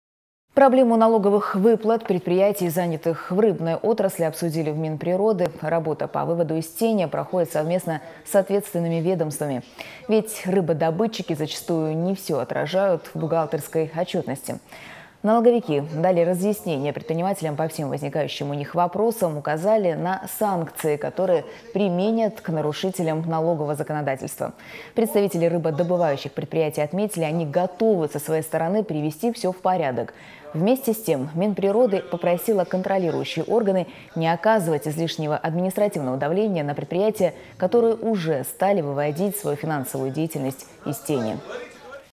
В Махачкале обсудили проблему налоговых выплат предприятий рыбной отрасли
Проблему налоговых выплат предприятий, занятых в рыбной отрасли, обсудили в Минприроды. Работа по «выводу из тени» проходит совместно с ответственными ведомствами. Ведь рыбодобытчики, зачастую, не всё отражают в бухгалтерской отчетности.